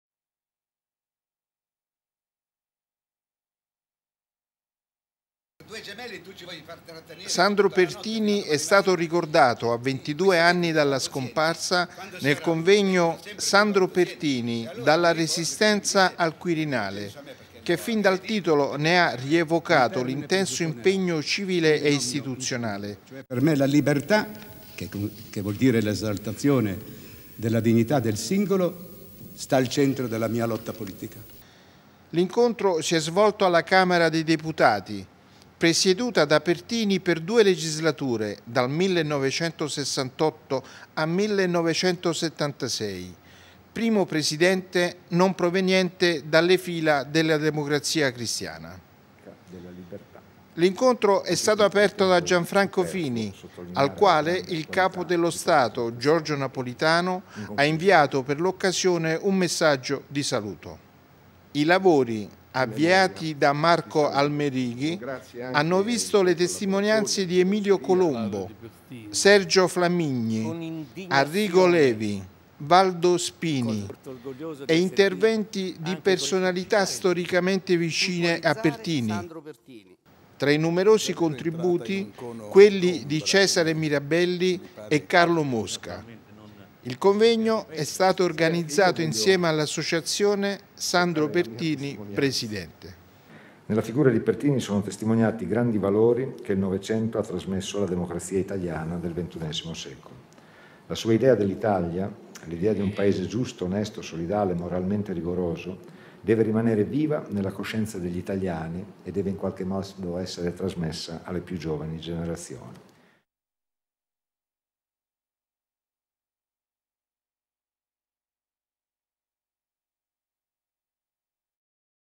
Camera, l'attualità di Sandro Pertini
Sandro Pertini è stato ricordato a ventidue anni dalla scomparsa nel convegno "Sandro Pertini dalla Resistenza al Quirinale" tenuto nella Sala della Lupa della Camera dei deputati. Introduzione di  Gianfranco Fini. Messaggio di saluto del Presidente della Repubblica, Giorgio Napolitano. Lavori aperti da Mario Almerighi, le testimonianze di  Emilio Colombo, Sergio Flamigni, Arrigo Levi, Valdo Spini. Tra i numerosi contributi, quelli di Cesare Mirabelli e Carlo Mosca. Ha concluso Adelmo Manna. Il convegno è stato organizzato insieme all'associazione "Sandro Pertini Presidente"  (20 marzo 2012)